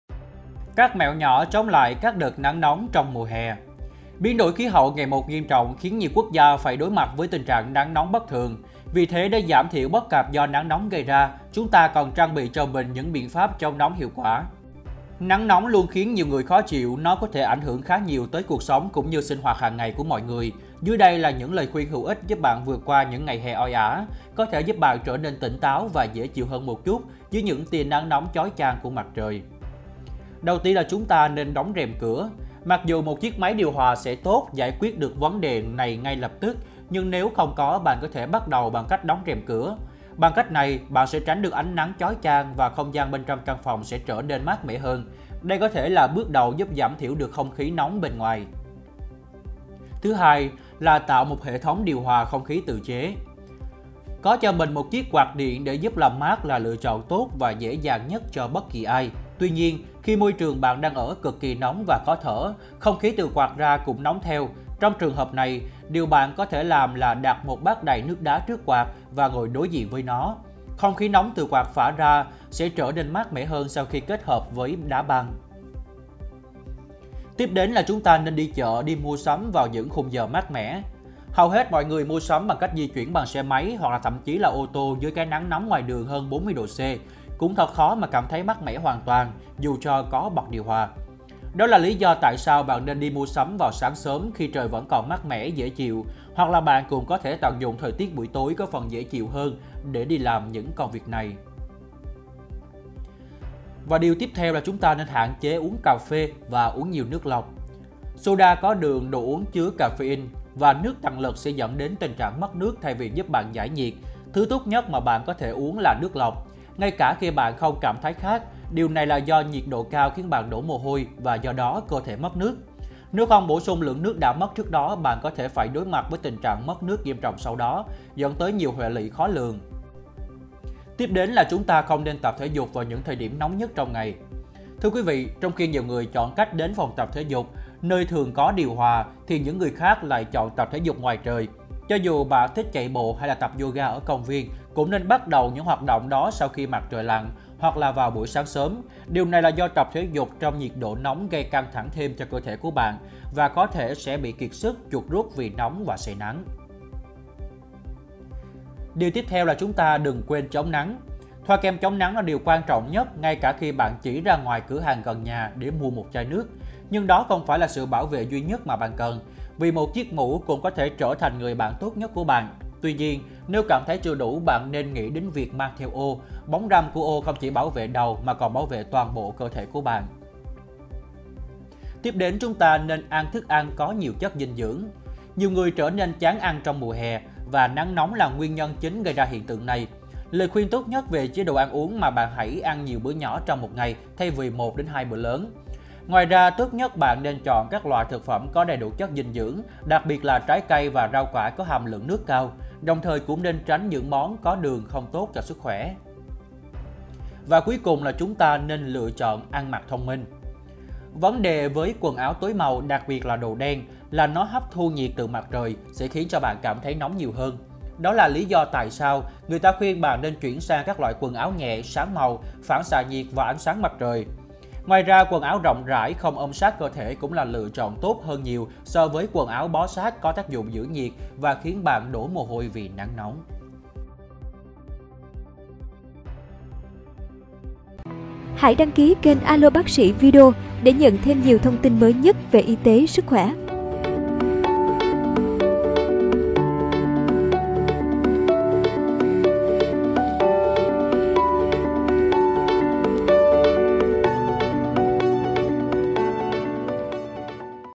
Mẹo chống lại các đợt nắng nóng trong mùa hè
Biến đổi khí hậu ngày một nghiêm trọng khiến nhiều quốc gia phải đối mặt với tình trạng nắng nóng bất thường. Vì thế, để giảm thiểu bất cập do nắng nóng gây ra, chúng ta cần trang bị cho mình những biện pháp chống nóng hiệu quả.👉 Tin tức COVID-19 mới nhất

MỌI GÓP Ý XIN GỬI VỀ:
Hợp tác nội dung:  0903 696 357